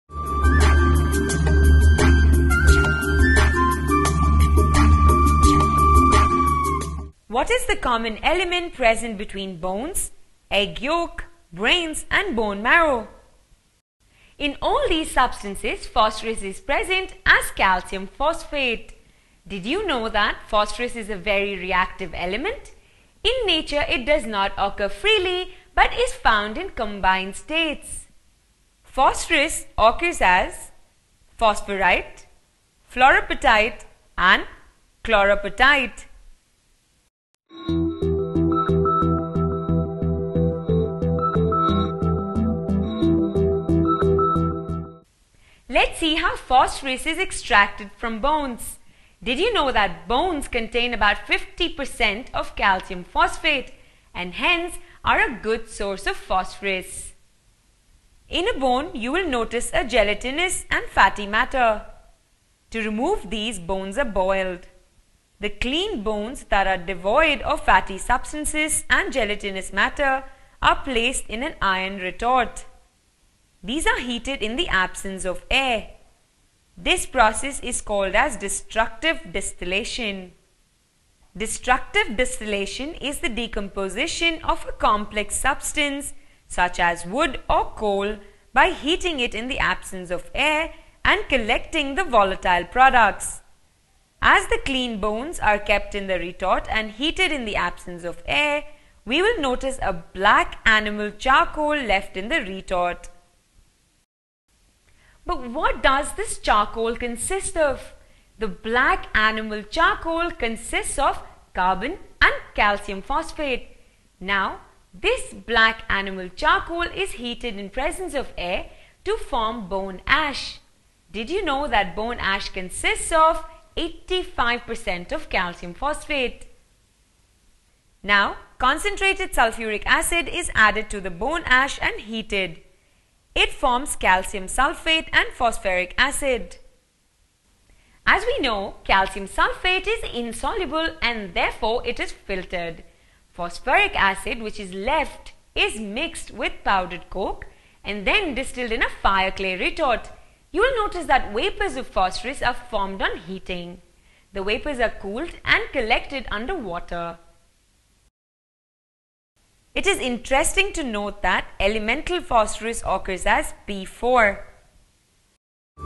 Phosphorus and its Compounds:-Occurrence of phosphorus-02
There are three main forms of phosphorus on earth: water, soil, and sediments. The gaseous state of air cannot contain phosphorus, unlike other matter cycles. When temperatures and pressures are normal, phosphorus is usually liquid. Take a look at the video and see where else we can find phosphorus .